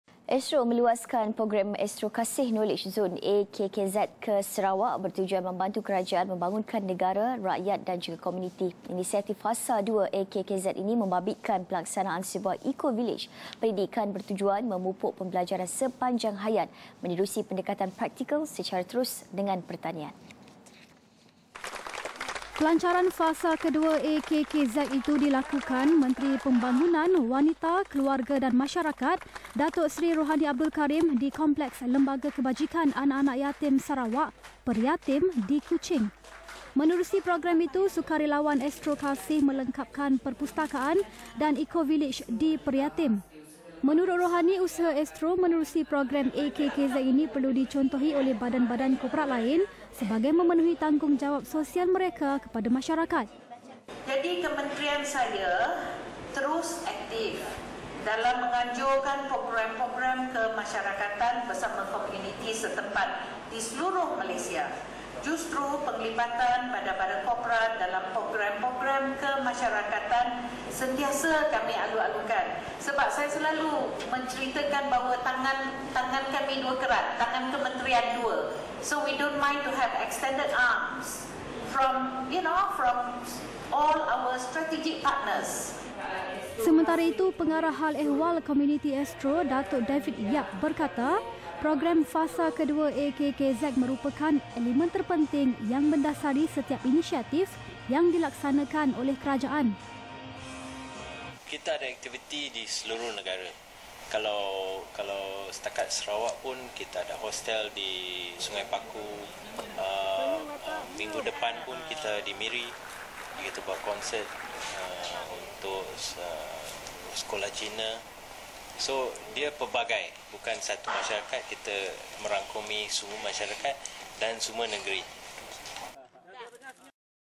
Astro Kasih Knowledge Zone fasa dua dilancarkan
Astro meluaskan program Astro Kasih Knowledge Zone (AKKZ) ke Sarawak bertujuan membantu kerajaan membangunkan negara, rakyat dan komuniti.

Inisiastif fasa dua AKKZ ini membabitkan pelaksanaan sebuah Eko Village pendidikan bertujuan memupuk pembelajaran sepanjang hayat menerusi pendekatan praktikal secara terus dengan pertanian.